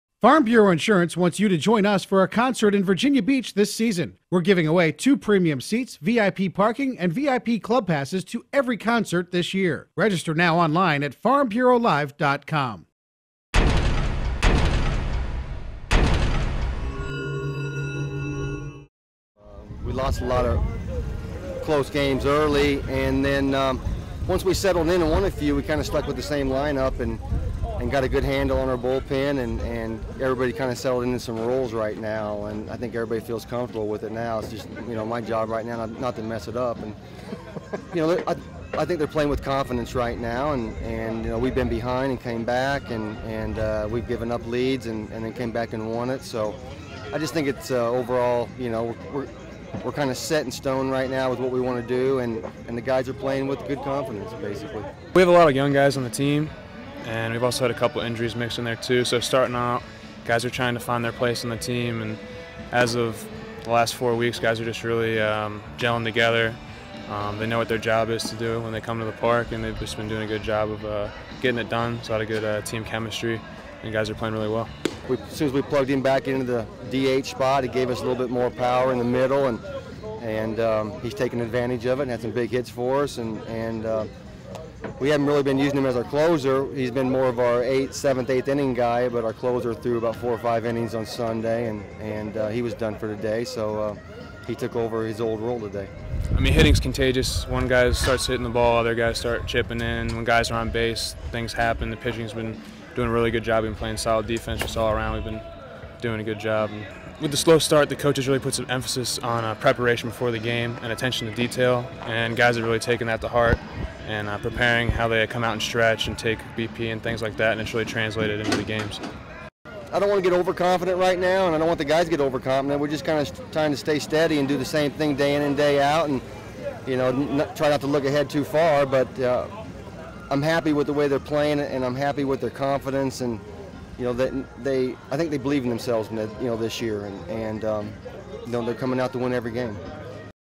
SpiderTVHD: Richmond Baseball Downs VCU
Billy Barber hit a two-run home run and picked up the save as Richmond defeated VCU 6-3 Wednesday at Pitt Field.